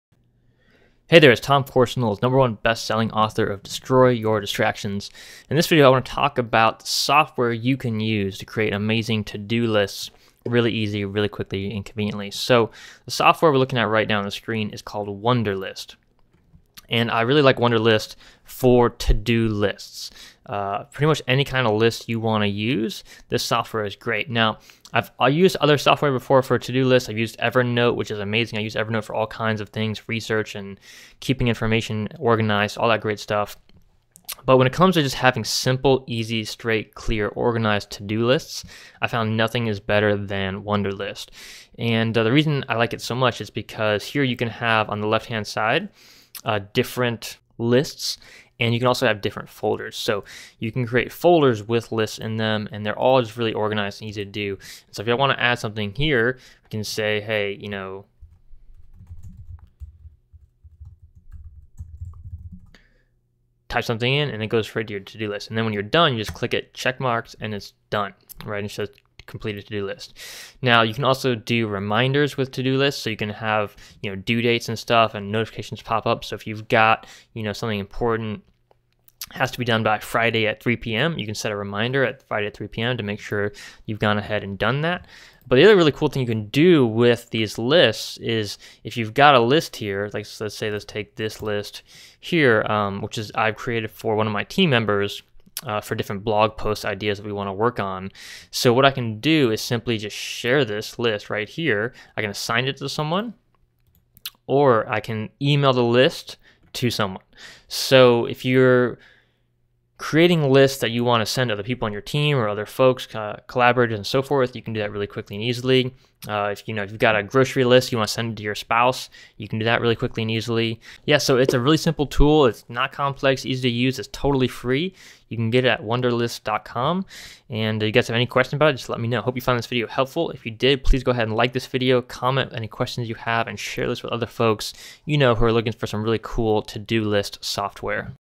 The Best Free To Do List Software: Wunderlist (Review + Tutorial)
In this video, Tom Corson-Knowles, #1 Amazon bestselling author of Destroy Your Distractions and founder of TCK Publishing shows you the best free to do list software out there: it's called Wunderlist!

★☆★ FREE ACCESS TO MY LATEST WEBINARS ★☆★

★☆★ GET YOUR BOOK PUBLISHED ★☆★

★☆★ SHARE THE INSPIRATION ★☆★